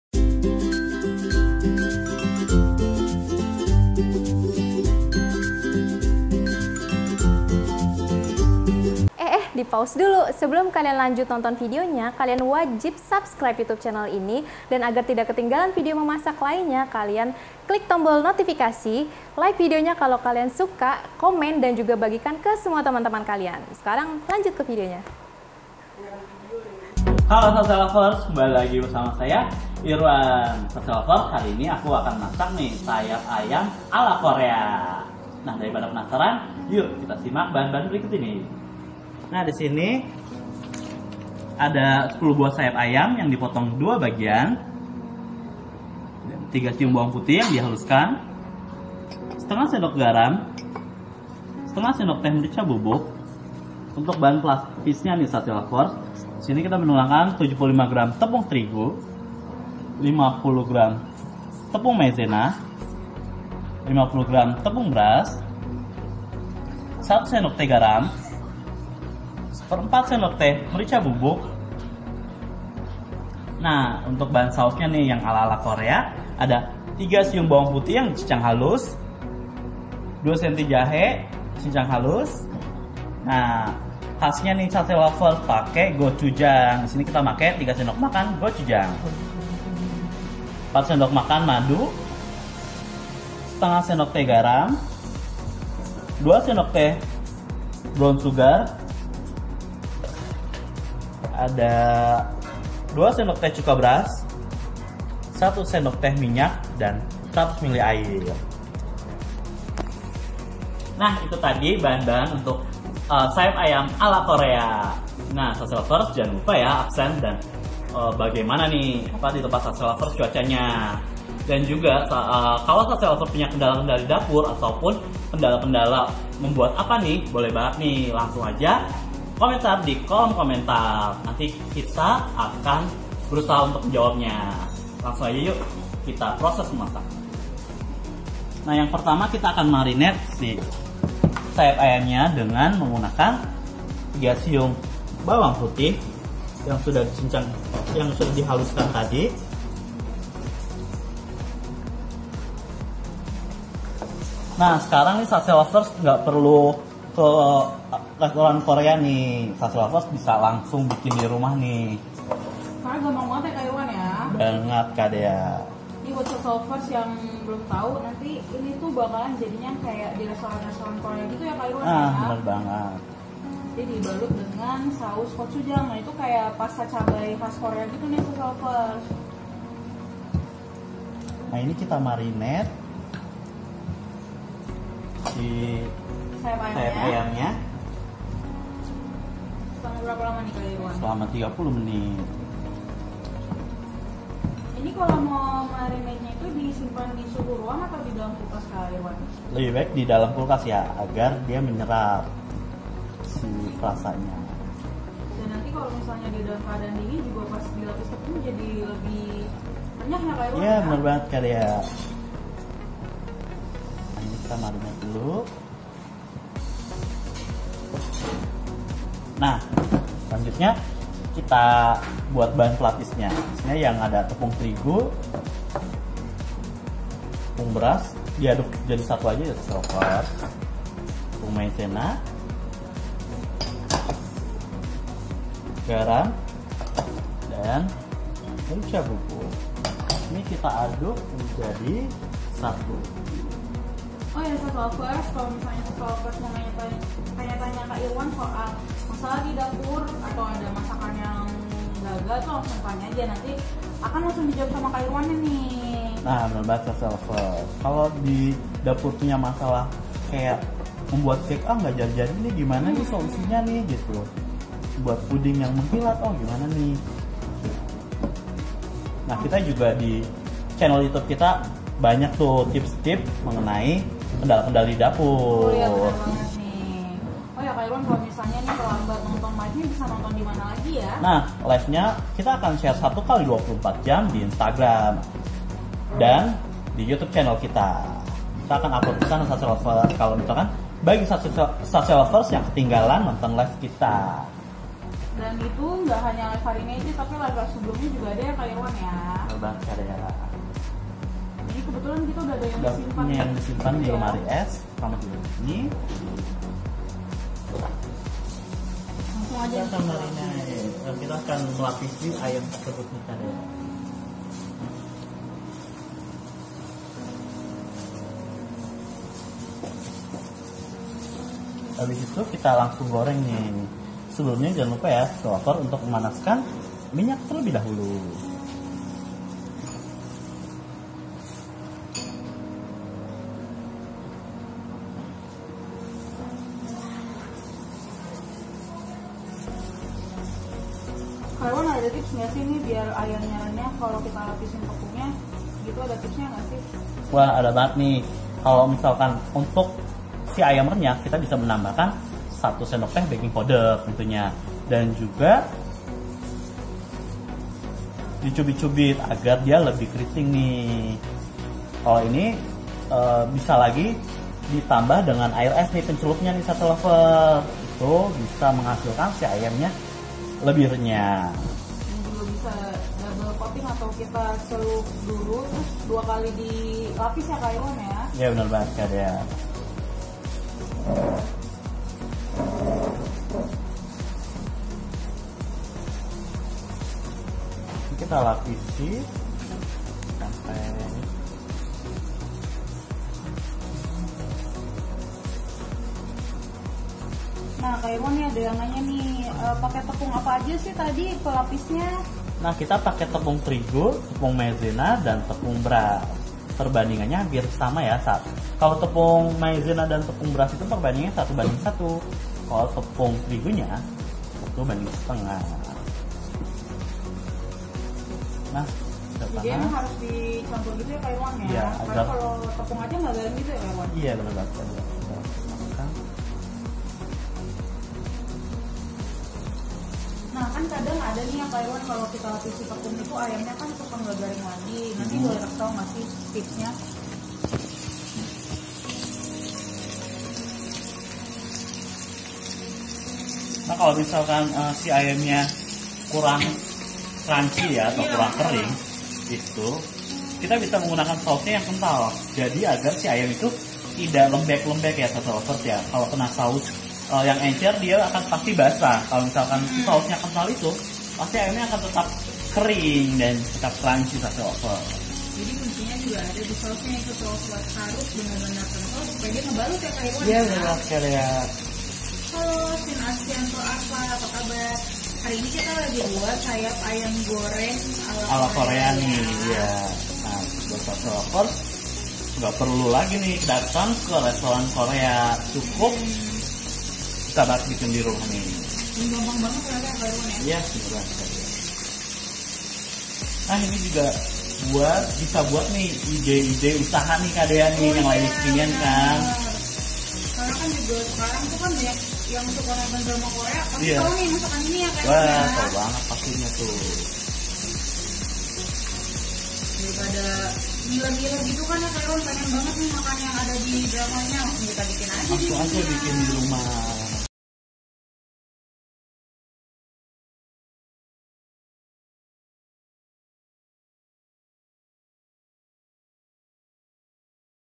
Resep Sayap Ayam Goreng Korea 1, Resep Mudah yang Bisa Dibuat di Rumah
00:01- intro
00:31- opening
00:37- bahan-bahan sayap ayam ala kore
02:46- cara membuat sayap goreng tepung

Produser:
Penulis Naskah:
Peraga:
Fotografer:

Resep Sayap Ayam Ala Korea 1, Resep Mudah yang Bisa Dibuat di Rumah